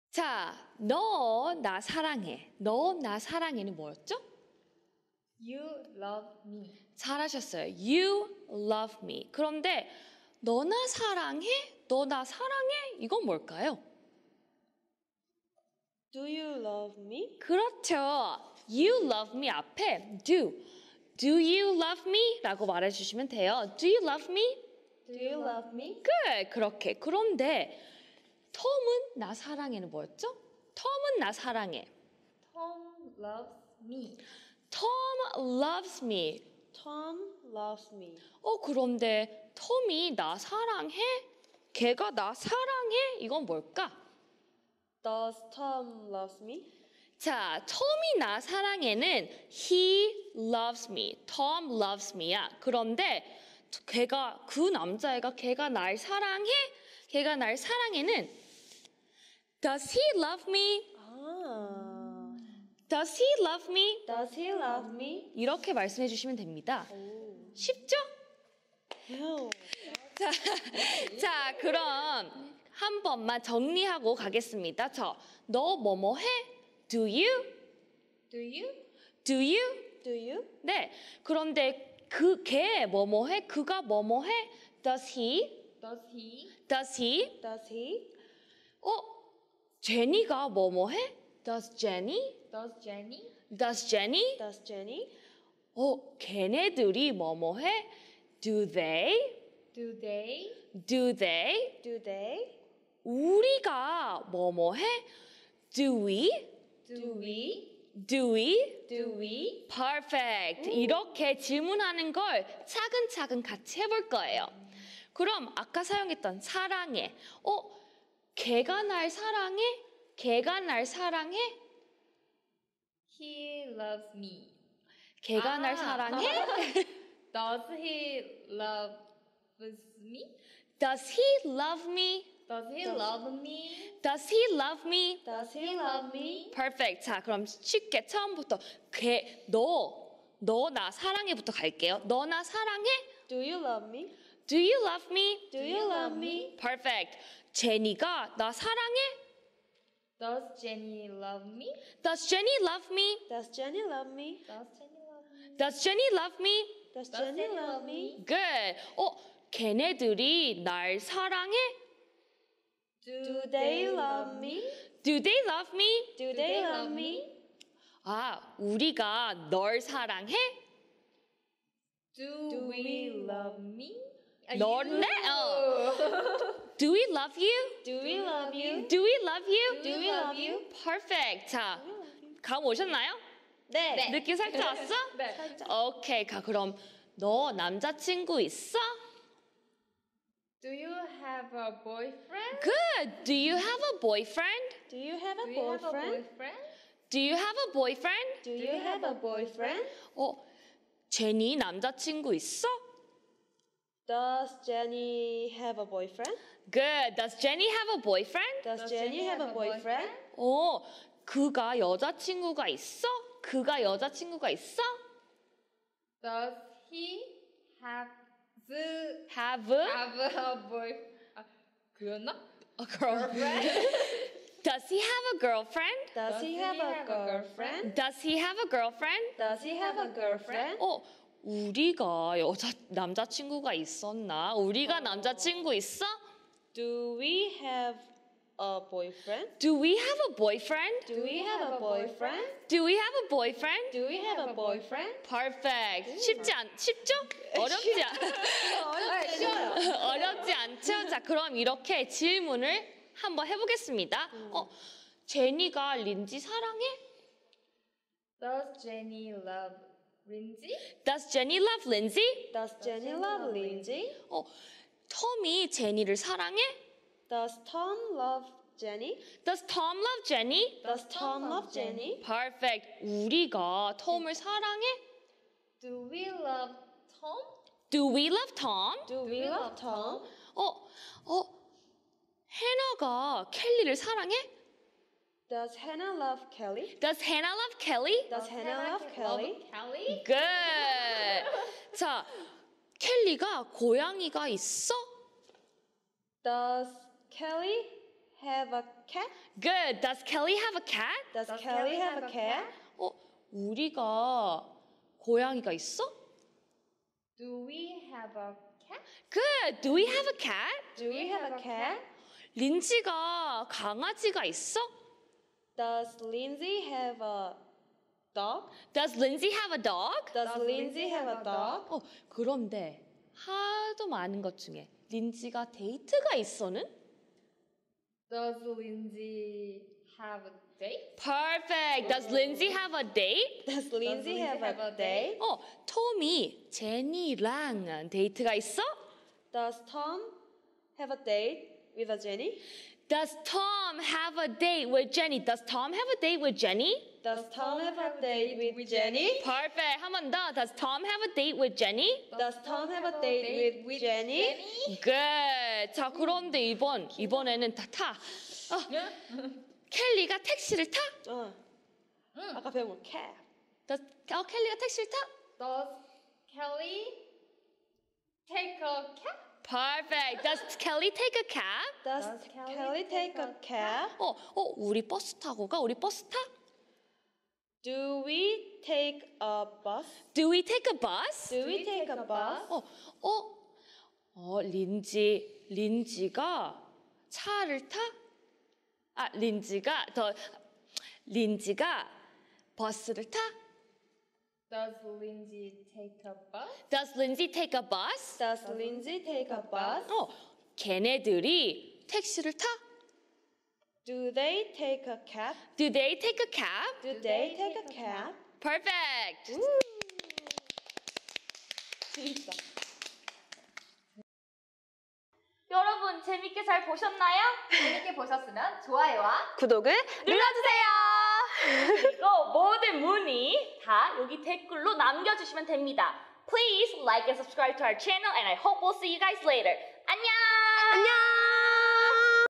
#2 [왕초보 영어회화] Ep1S1 (2/3) 영어로 질문하기 - 영어 질문 어렵지 않아요~ | 학생보다 먼저 대답해보세요!~★ | 잉머 | 원어민 표현
이제 "영어로 질문을 하는 연습"을 해봐요~
어려울 것 같다구요? 학생들과 함께 천천히 시작해봐요!

그녀들의 달콤한 영어 배틀이 시작된다!"
린지 쌤과  함께 하는 왕초보 학생들의  유쾌한 영어회화 초보 탈출기

입으로 말하면 효과가 100배가 됩니다!~